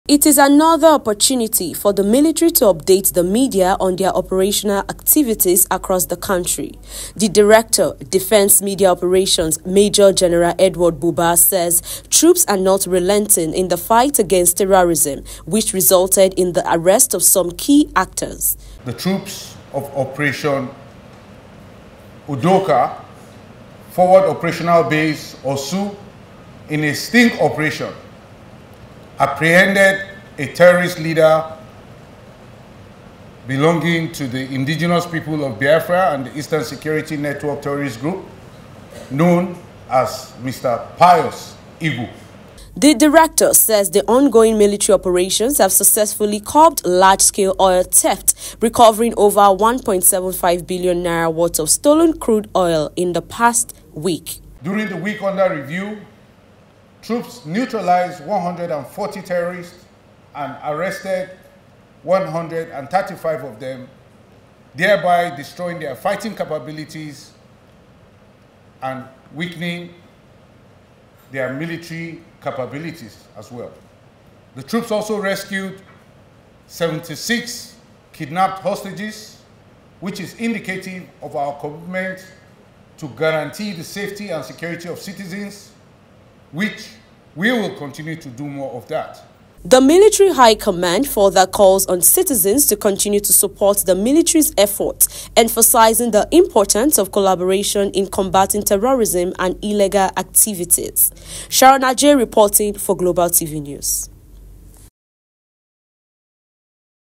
FIGHT AGAINST TERRORISM:TROOPS ARREST IPOB FOUNDING FATHER.
The Defence Headquarters has confirmed the arrest of one of the founding fathers of the indigenous people of Biafra, Pius Iguh.